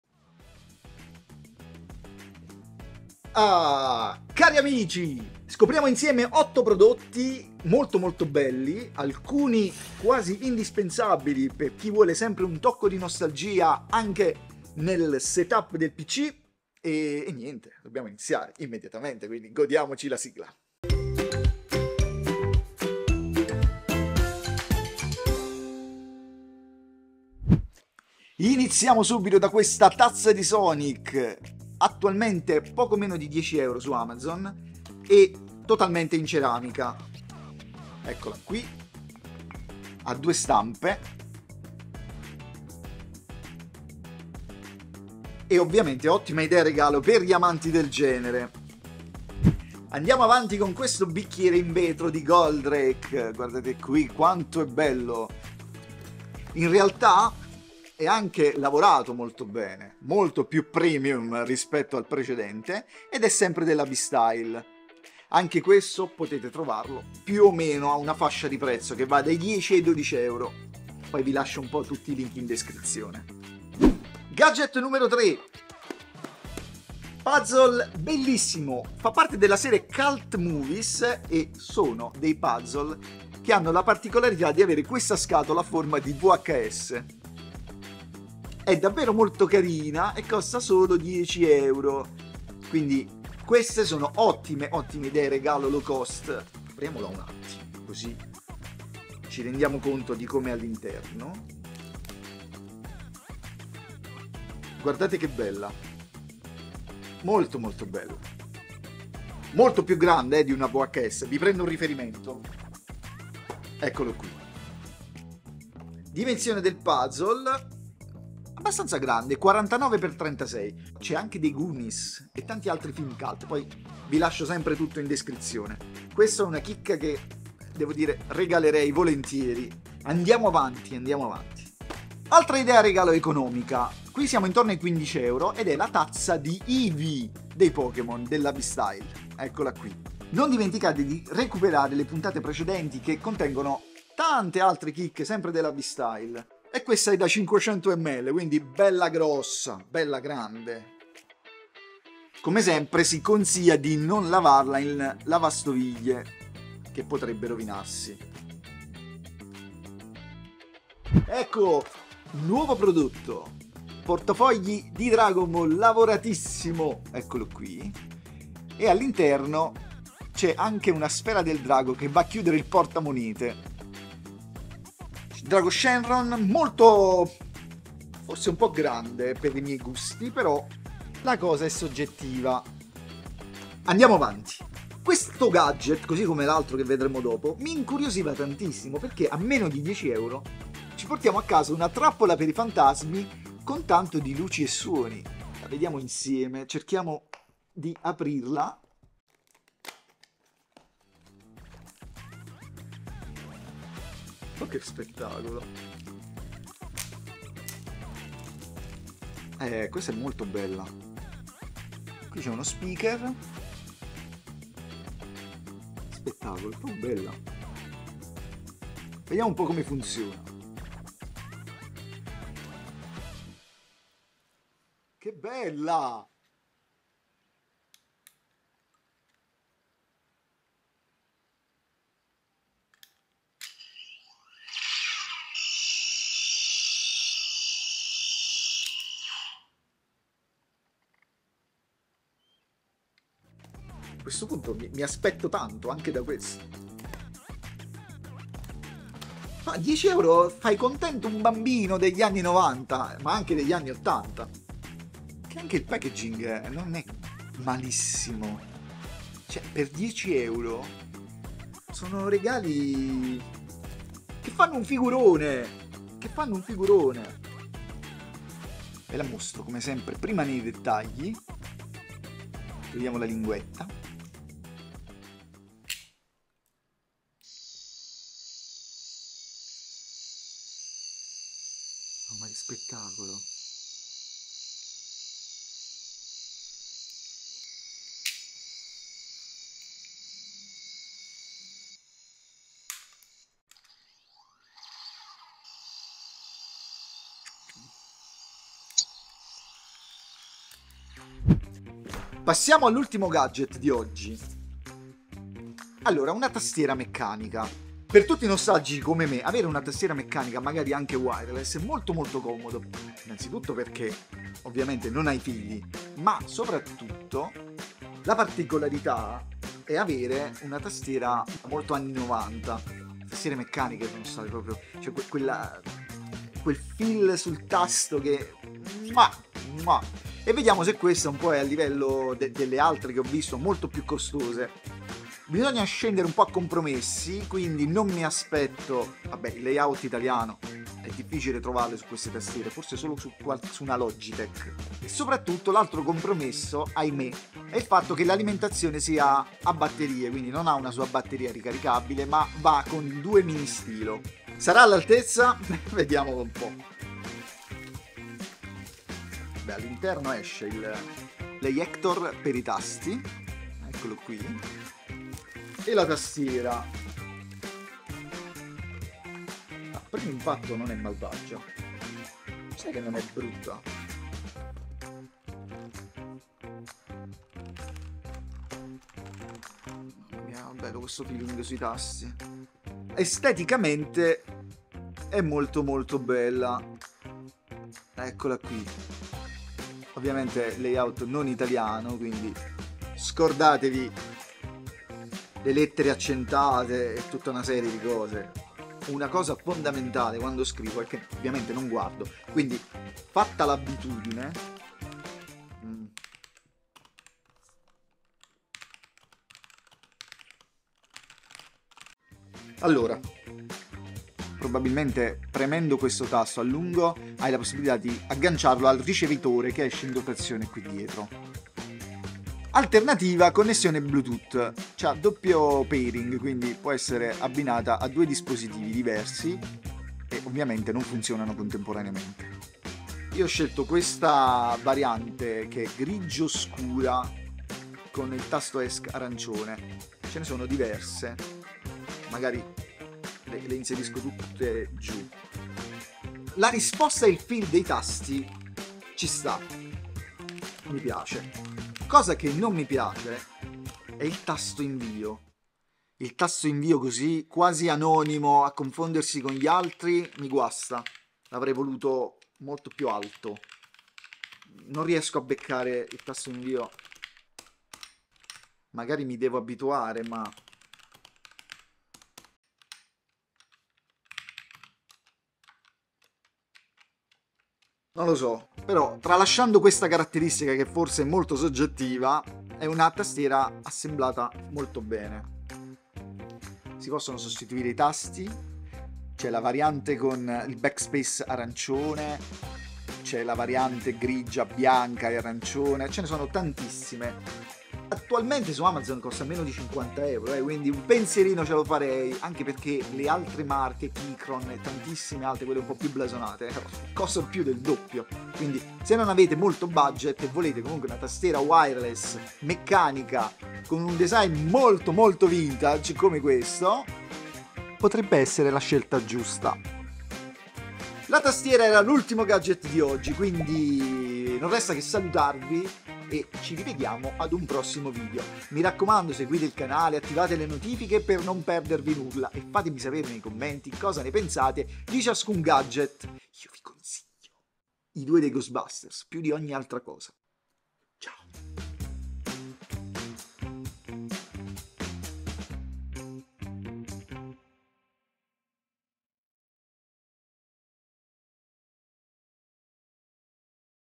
Otto idee regalo super economiche per veri Nerd Nostalgici!
Capitoli:
00:00 Intro
00:28 Tazza Sonic Abystyle
00:48 Bicchiere in Vetro Goldrake Abystyle
01:14 Puzzle Clementoni Ritorno al Futuro
02:12 Tazza Pokemon EEVEE Abystyle
02:42 Portafogli Dragon Ball Z Abystyle
03:05 Ghost Trap Ghostbusters
04:03 Rilevatore / Meter Ghostbusters
05:01 Tastiera Meccanica MAGIC-REFINER RK68

Link Acquisti :
- Tazza Sonic
- Bicchiere Goldrake
- Puzzle Clementoni
- Tazza EEVEE

Nella quarta puntata di "Gadget per veri nostalgici"...Otto idee Regalo low Cost!
Tazze dei Pokemon, Bicchieri di Goldrake Grandizer (Goldorak), Puzzle di film Cult come Ritorno al futuro, un rilevatore e una Trappola dei Ghostbusters e per concludere una tastiera meccanica della Magic-Refiner che costa meno di 50 euro!